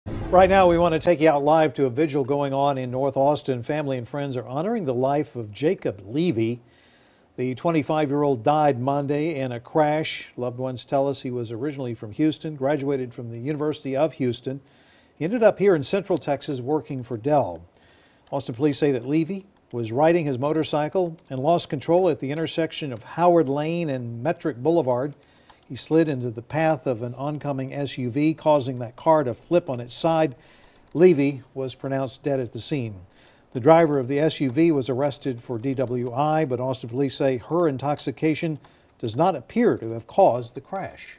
Vigil held in north Austin honoring the life of Jacob Levy
APD says Mitchell Jacob Levy, 25, was riding his motorcycle toward the 1600 block of West Howard Lane around 9:23 p.m.